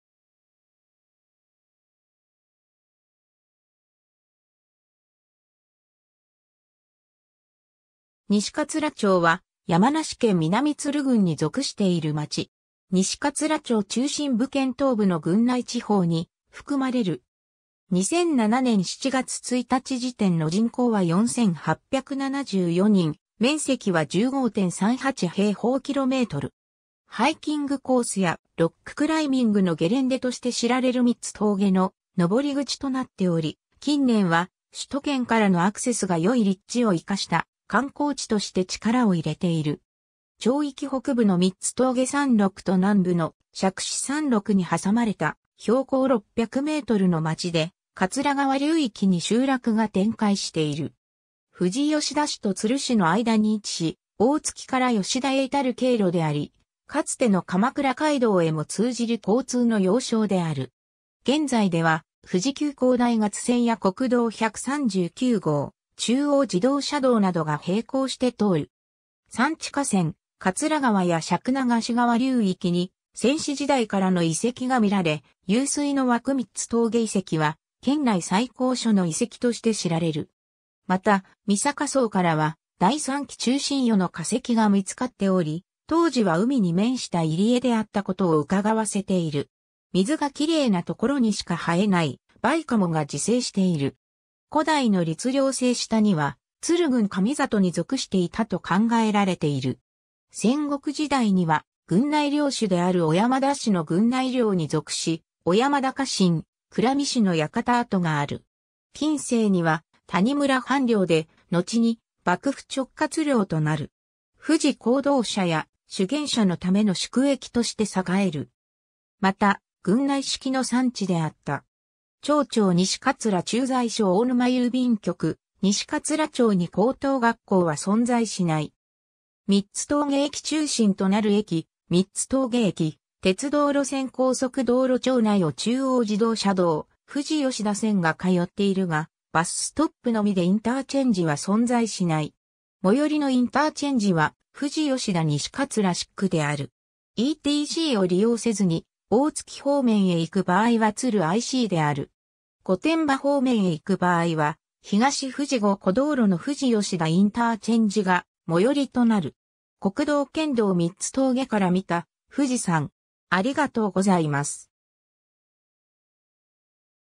西桂町
西桂町（にしかつらちょう）は、山梨県南都留郡に属している町。
 西桂町中心部 県東部の郡内地方に含まれる。
2007年（平成19年）7月1日時点の人口は4,874人、面積は15.38km2。
 ハイキングコースやロッククライミングのゲレンデとして知られる三つ峠の登り口となっており、近年は首都圏からのアクセスが良い立地を生かした観光地として力を入れている。
 町域北部の三つ峠山麓と南部の杓子山麓に挟まれた標高600mの町で、桂川流域に集落が展開している。
富士吉田市と都留市の間に位置し、大月から吉田へ至る経路であり、かつての鎌倉街道へも通じる交通の要衝である。
現在では富士急行大月線や国道139号、中央自動車道などが平行して通る。
 山地 河川 桂川や柄杓流（しゃくながれ）川流域に先史時代からの遺跡がみられ、湧水の湧く三つ峠遺跡は県内最高所の遺跡として知られる。
 また、御坂層からは第三紀中新世の化石が見つかっており、当時は海に面した入り江であったことを窺わせている。
水が綺麗なところにしか生えないバイカモが自生している。
 古代の律令制下には都留郡加美郷に属していたと考えられている。
 戦国時代には郡内領主である小山田氏の郡内領に属し、小山田家臣・倉見氏の館跡がある。
 近世には谷村藩領で、のちに幕府直轄領となる。
富士講道者や修験者のための宿駅として栄える。
また、郡内織の産地であった。
 町長 西桂駐在所 大沼郵便局 西桂町に高等学校は存在しない。
 三つ峠駅 中心となる駅：三つ峠駅 鉄道路線 高速道路 町内を中央自動車道富士吉田線が通っているが、バスストップのみでインターチェンジは存在しない。
 最寄りのインターチェンジは富士吉田西桂SICである。
ETCを利用せずに大月方面へ行く場合は都留ICである。
 御殿場方面へ行く場合は東富士五湖道路の富士吉田ICが最寄となる。
 国道 県道 三つ峠から見た富士山